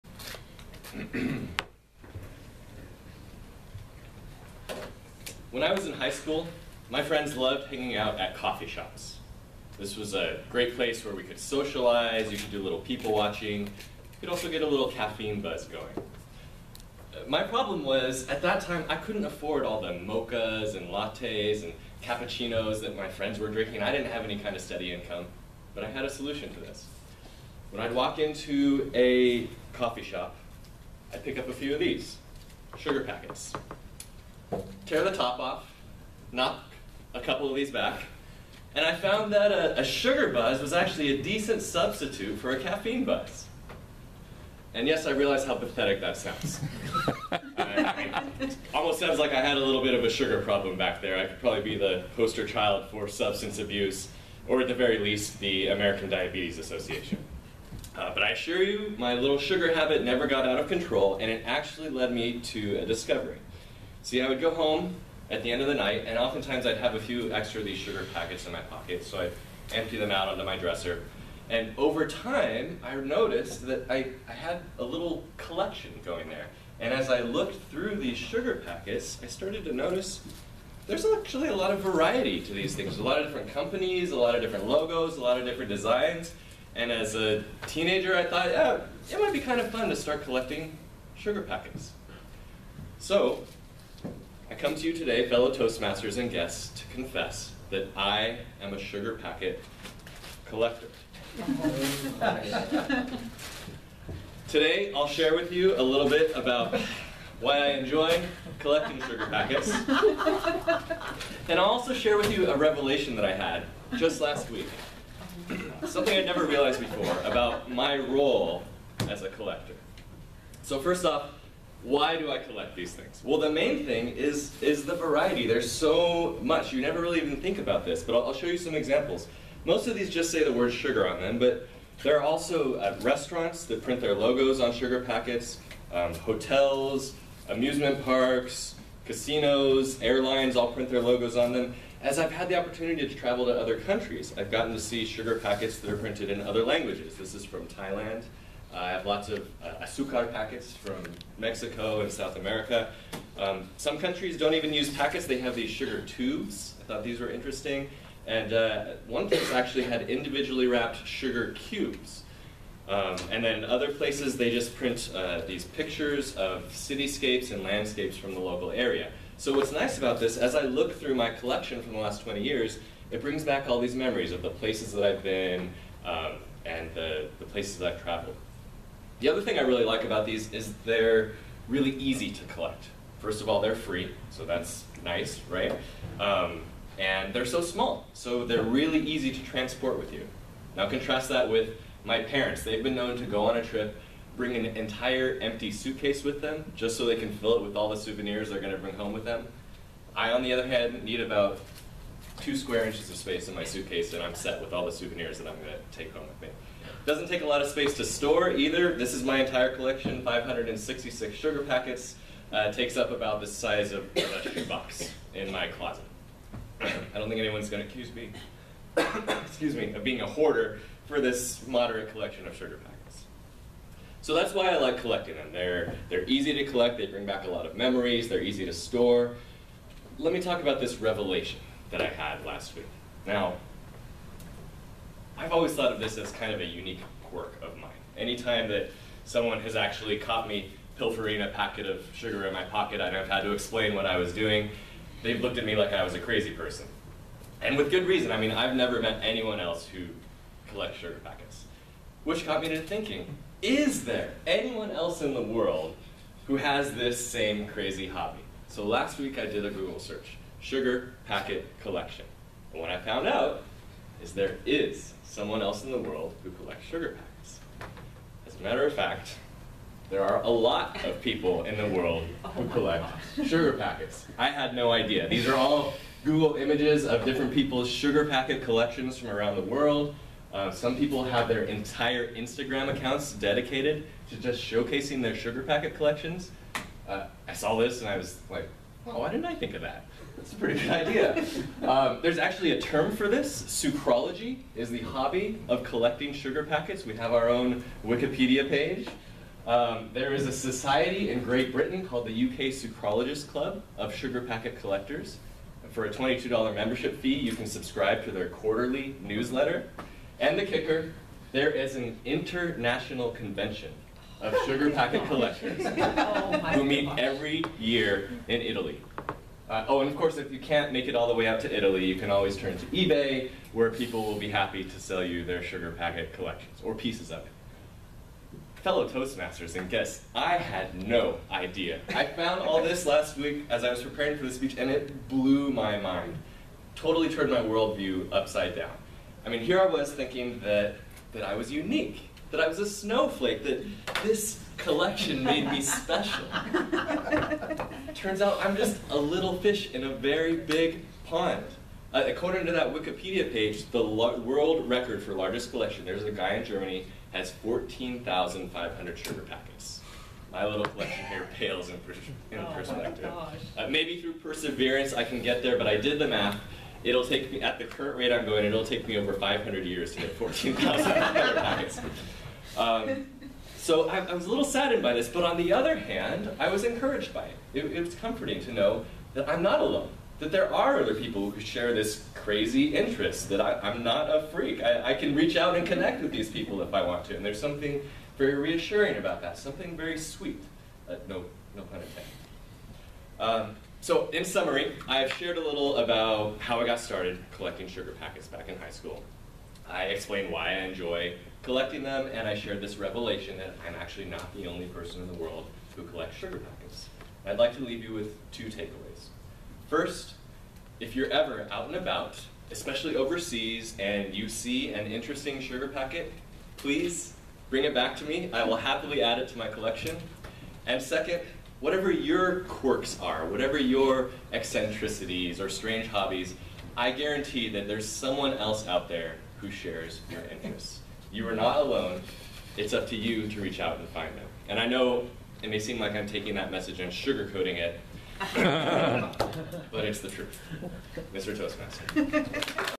Philip Williams: How Sweet It Is
Philip showcases his odd collection of sugar packets (Pathways Level 3 project: Present with Visual Aids, Jan 23 2019)